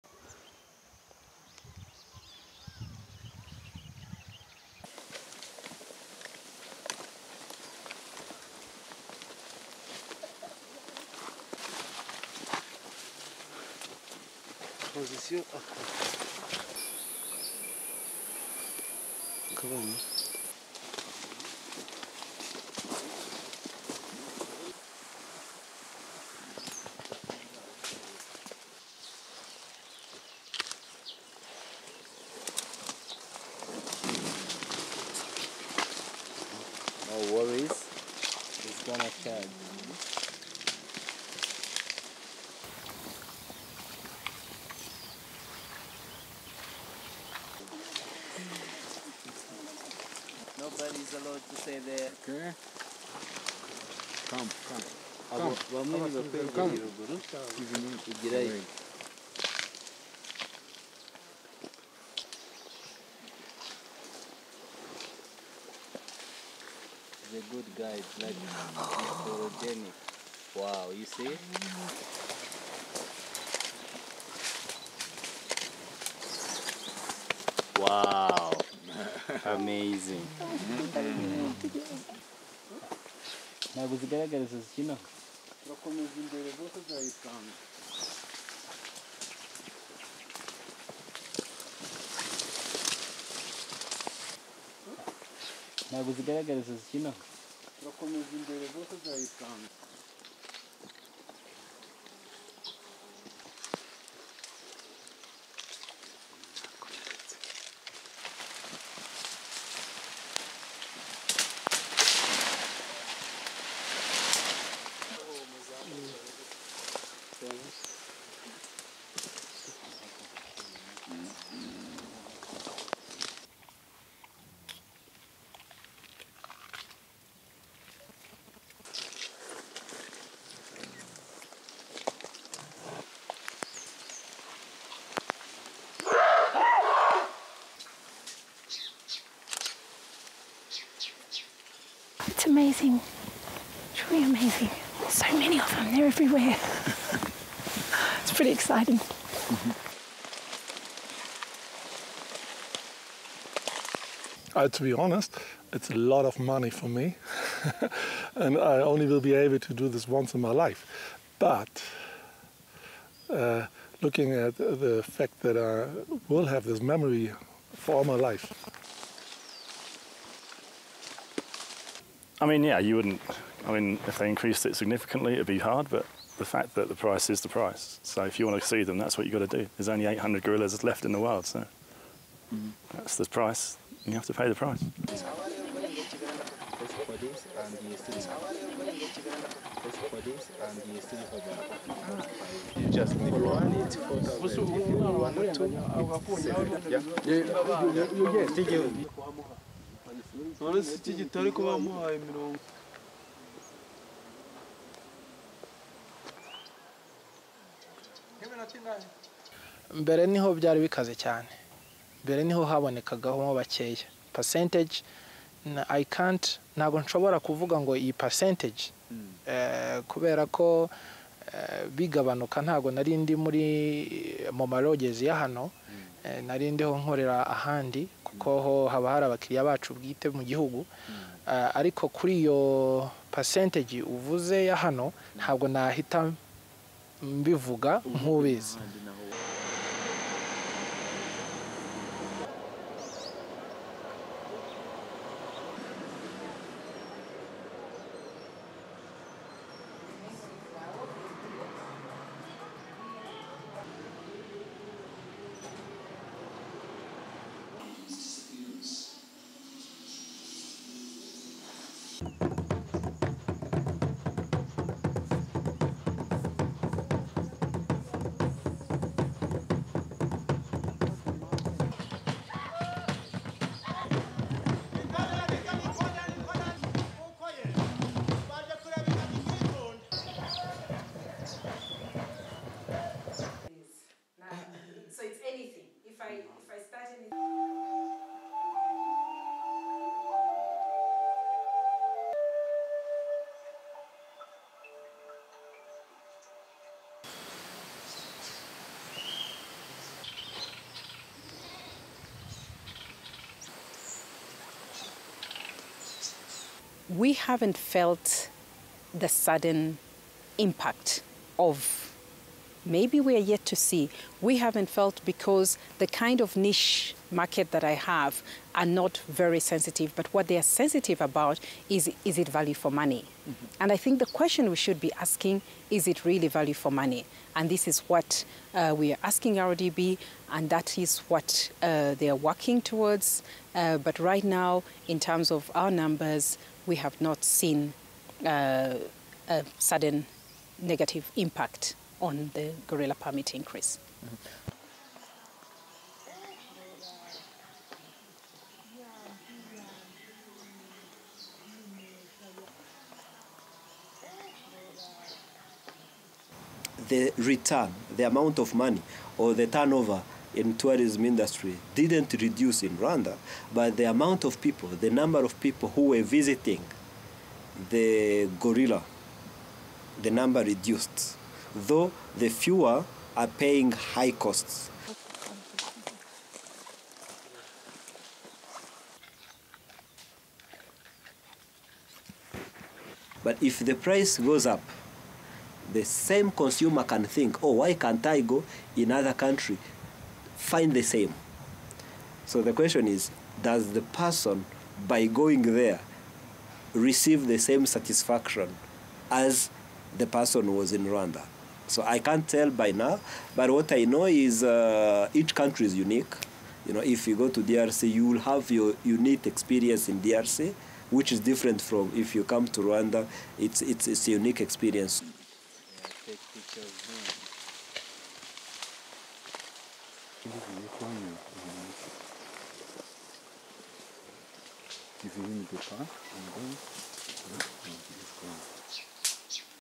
Gorilla tourism discount after recent rise kept tourists away
(28 Sep 2018) LEADIN:
Gorilla tourism is an important income source for Rwanda, but an increase in permit fees for safaris has meant a steep decline in visitors.
Now fees have been reduced by 30 percent for the low season in the hope that visitors will return to the mountains to view the primates up close.
STORYLINE:
Deep in Rwanda's steep-sloped forest, the mountain gorillas look both endearing and intimidating.
The eastern gorilla, the world's largest living primate, was listed as critically endangered in 2016 by the International Union for the Conservation of Nature, or IUCN.
The Mountain Gorilla subspecies (Gorilla beringei beringei), has been listed as critically endangered since 1996, but is making a recovery according to the World Wildlife Fund (WWF).
In May 2018 the WWF said mountain gorilla numbers have increased to 604 from an estimated 480 in 2010 in the Virunga Massif, a hilly area encompassing parts of Rwanda, Uganda and Congo.
In Rwanda' s Volcano National Park tourists routinely trek into the undergrowth to see gorillas in their natural habitat.
There are no barriers and no enclosures, and the apes, many of whom are accustomed to people, sometimes move past first-time visitors frozen in place, their awe mingling with apprehension.
The guides who lead tourists to see habituated gorillas ask them to stay at least 23 feet (seven meters) away from the animals. They also say: Don't point, speak softly, don't cough or sneeze in the animals' direction and if a gorilla approaches, crouch down, don't make eye contact and make a low sound similar to that of clearing the throat, which gorillas use to express friendliness.
Increasing numbers of tourists are heading to Rwanda's Volcanoes National Park, fueling an industry seen as key to the welfare of the critically endangered subspecies as well as the national economy.
"It is amazing, really amazing. There's so many of them. They are everywhere. It 's pretty exciting." says Catrina, a tourist from Ireland.
The tourists are visiting a family group of around 30 gorillas known as the Igisha group, which consists of 5 silverbacks (males), 7 adult females, 10 juveniles and 8 infants, according to the International Gorilla Conservation Programme in August 2018.
In Rwanda, conservation is big business, tourism is the country's top foreign exchange earner.
But in May 2017 the government doubled the price of a permit to visit the gorillas from $750 to $1500, making the fee the highest in the region, a similar fee costs $600 in Uganda, and $400 in DR Congo.
Diege Joost, a tourist from Germany, was happy to pay full price, as it was a once in a lifetime experience.
"To be honest, it is a lot of money for me. I will only be able to do this once in my life, but looking at the fact that I will have this memory for all my life."
British tourist Mark Cannally agrees: "The fact (is) the price is the price, if you want see them, that's what you've got to do. There's only 800 gorillas left in the world, so that's the price and you have to pay the price."
Rwanda sells 96 gorilla tracking permits per day — the highest in the region while the competitor Uganda sells 64 permits daily.
However following a drop in tourist numbers, the Volcanoes National Park has decided to reduce the fee during November to May to $1050, starting November 2018.
To qualify for the discount visitors must spend at least three days in the country and visit two additonal national parks, Nyungwe forest national park and Akagera national park, which have added costs, according to the Volcanoes National Park website.